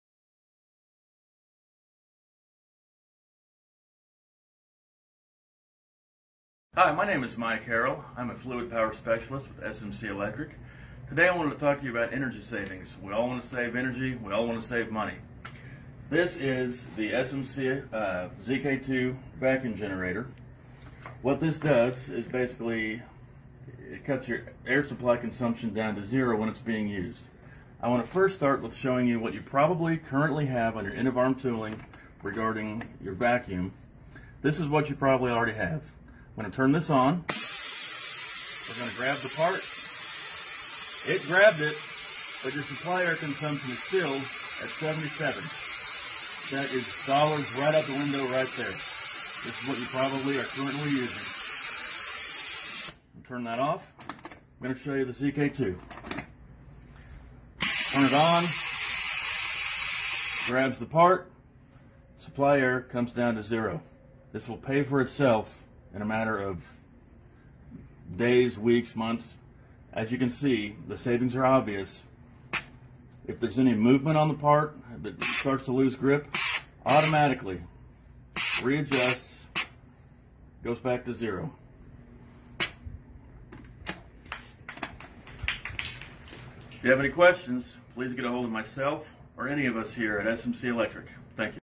Reduce Air Consumption and Save Money with this Vacuum Generator
The SMC Vacuum Generator is an extremely energy efficient vacuum generator that cuts your air consumption as much as 90%. It is light weight enough to be used on your end of arm tooling and can easily be interchanged with them current vacuum system you are using.